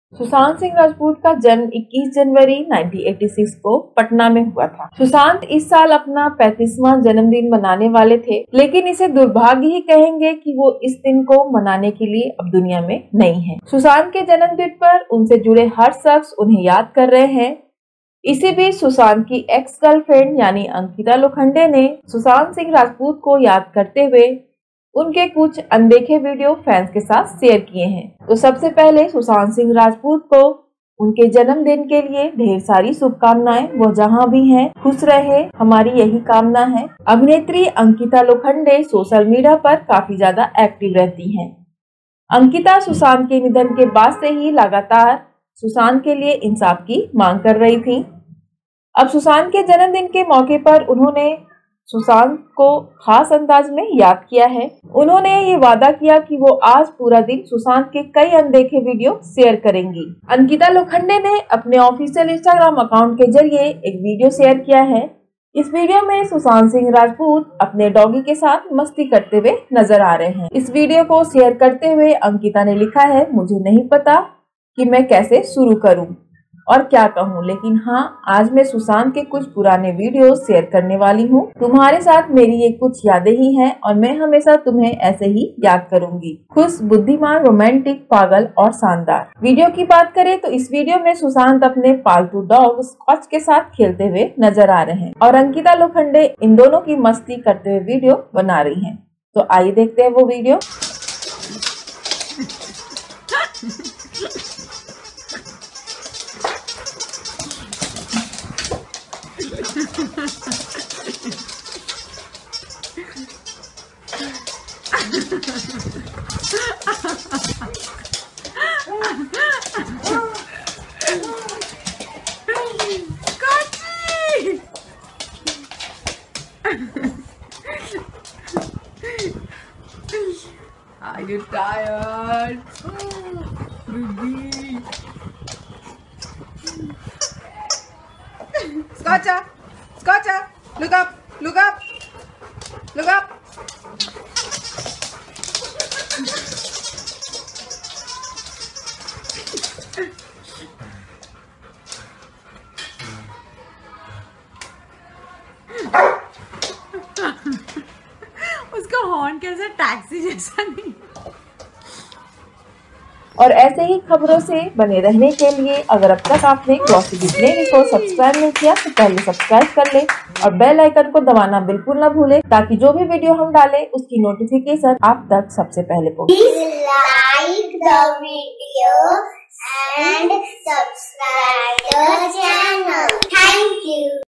Ankita Lokhande shares unseen videos of Sushant Singh Rajput playing with his pet dog Scotch.
Ankita Lokhande shares unseen videos of Sushant Singh Rajput playing with his pet dog Scotch, dancing to Shah Rukh Khan’s song
Actress Ankita Lokhande who is a former girlfriend of late actor Sushant Singh Rajput took to her social media handle and shared two unseen videos of the latter. In one video, Sushant can be seen being playful with his pet dog Scotch. In another video, he can be seen dancing to Shah Rukh Khan's song trying to imitate the superstar.
Posting the video where Sushant can be seen running around the house with his pet dog behind him.